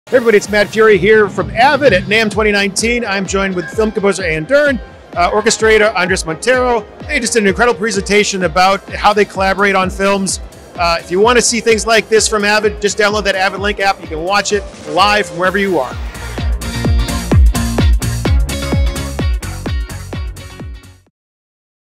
Anne Dern and Andrés Montero at NAMM 2019
Composer Anne Dern and orchestrator Andrés Montero discussed how they collaborate on scoring films.